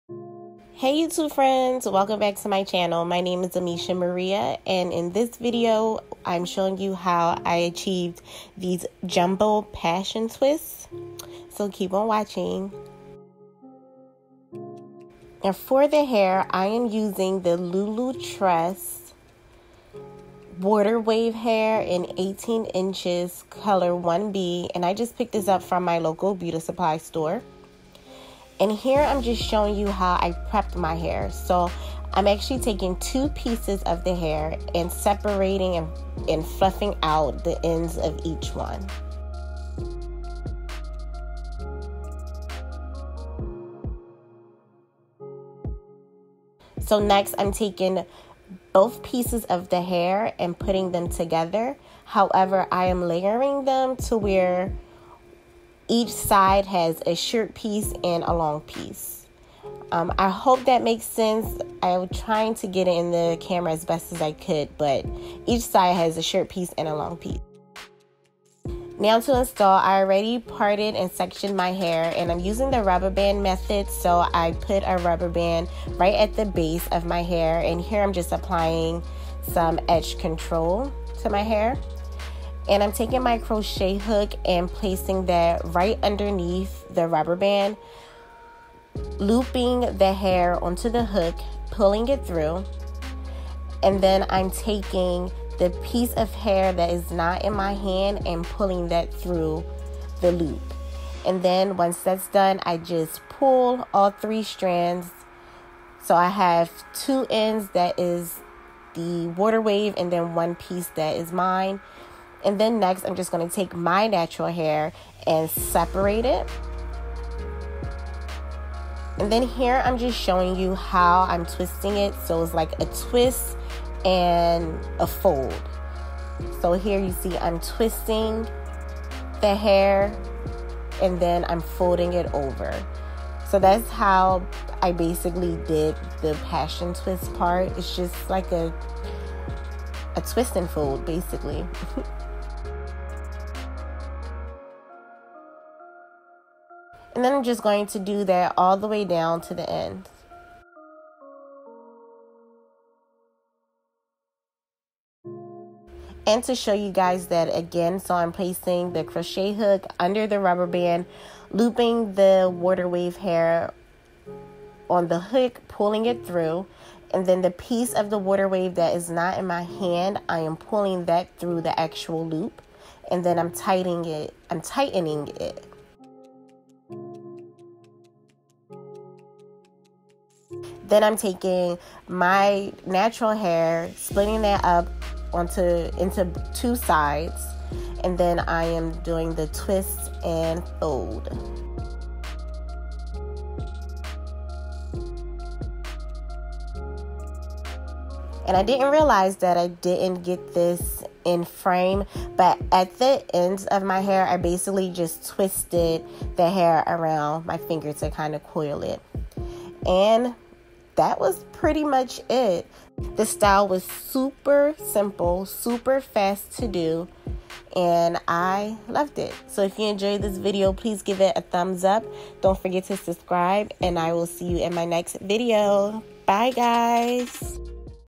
Easy Jumbo Passion Twists | Rubber Band Method
Hey Youtube Friends!!

I think video I show you how I achieved these Easy Jumbo Passion Twists using the Rubber Band Method. This style was very cute and long lasting.

Used 6 Packs of Water Wave hair.